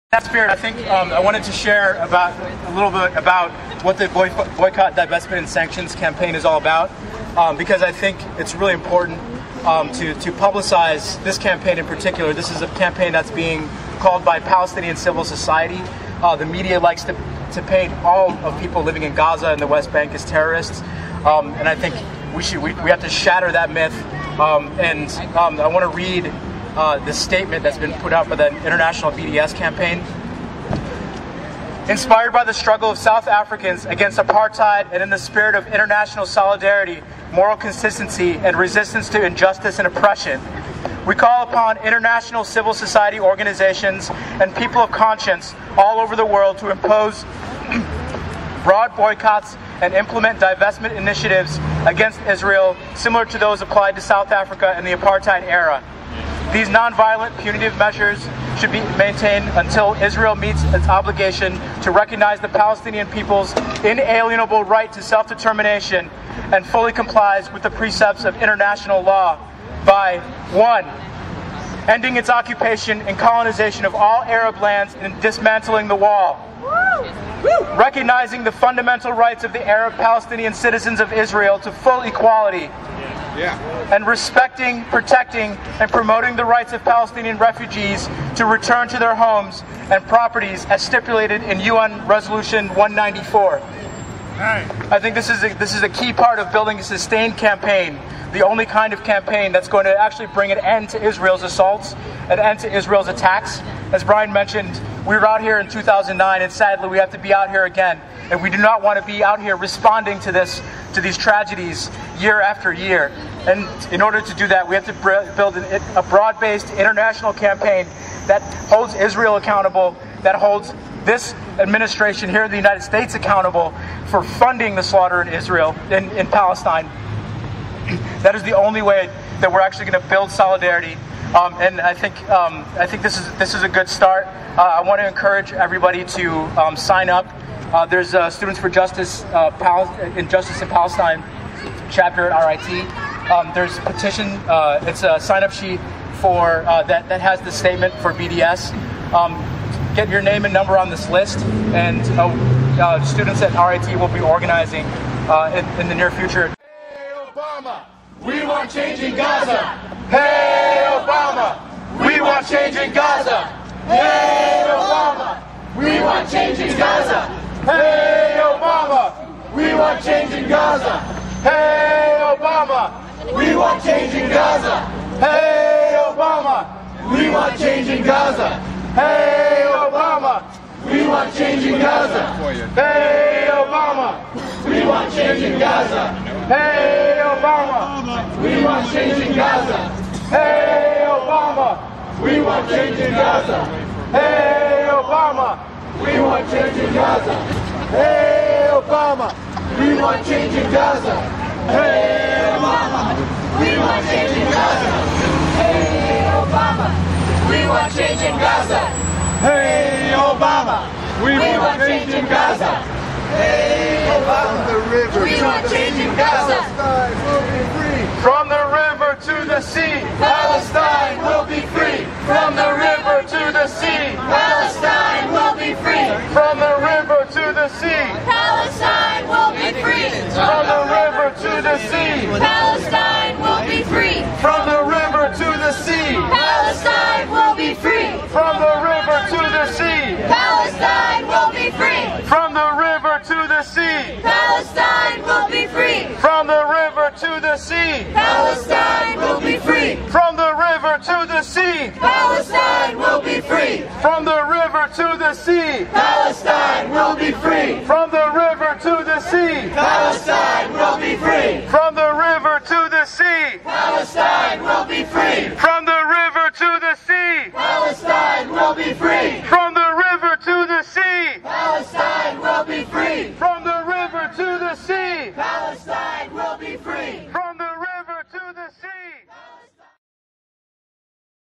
Ream Kidane speaks on the Israeli attack on Gaza
Ream Kidane of Rochester Against War speaks on Israeli attack on Gaza (November 17, 2012)